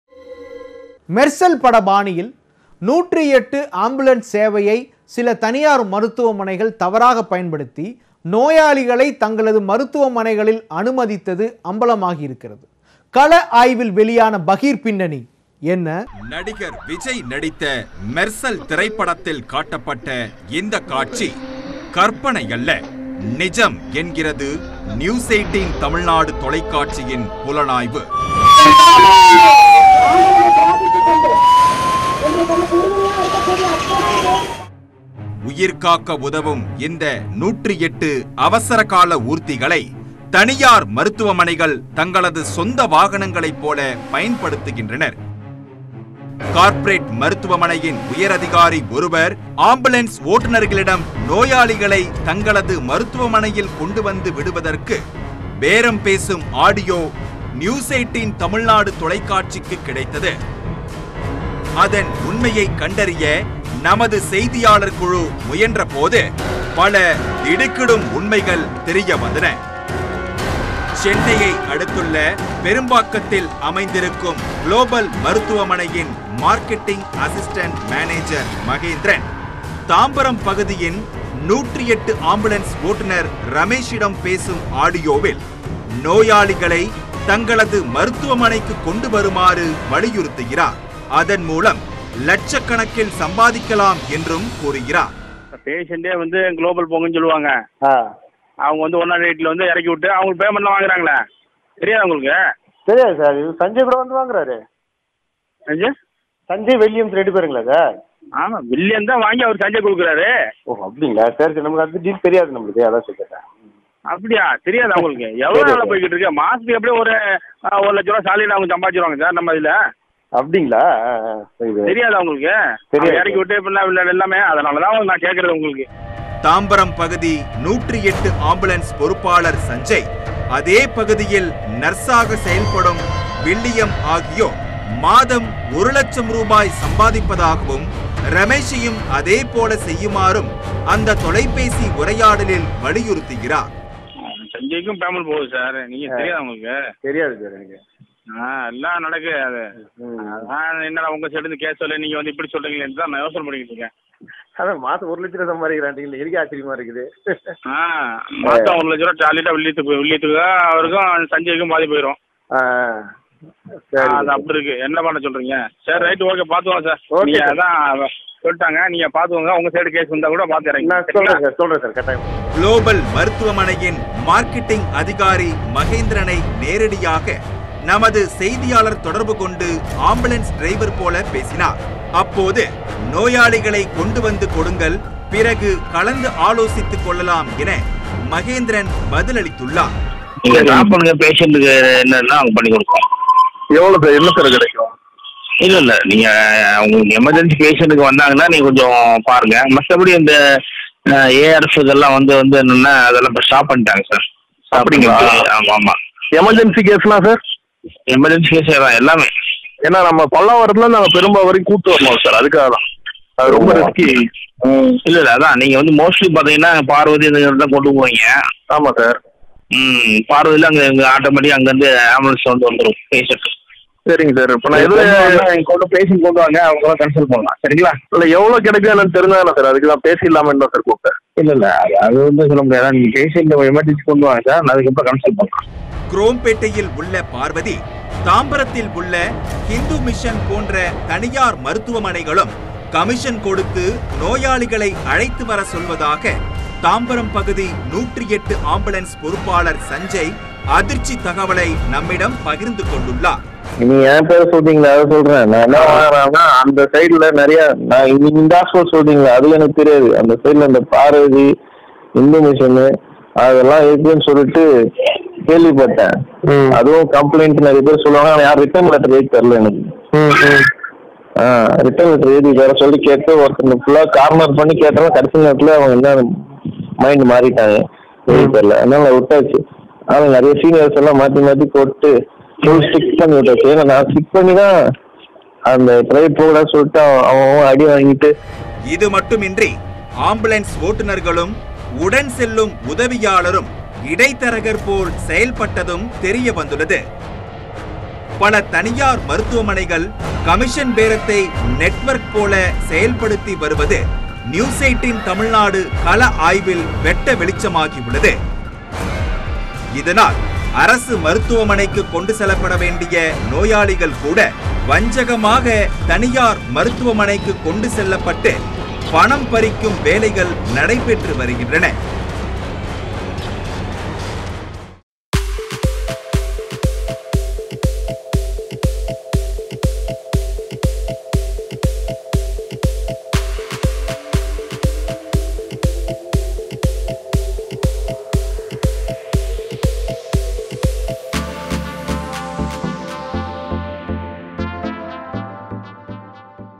Crime Time: மெர்சல் பட பாணியில் 108 ஆம்புலன்ஸ் ஊழியரிடம் தனியார் மருத்துவமனை பேரம்
அவசர தேவைகளுக்காக அரசின் சார்பில் இலவச சேவையாக  108 ஆம்புலன்ஸ் பயன்படுத்தப்படுகிறது. ஆனால் சில தனியார் மருத்துவமனை இதனைத் தவறாகப் பயன்படுத்தி நோயாளிகளை தங்களது மருத்துவமனைகளில் இறக்கிவிட வலியுறுத்தும் திடுக்கிடும் தகவல் வெளியாகியிருக்கிறது.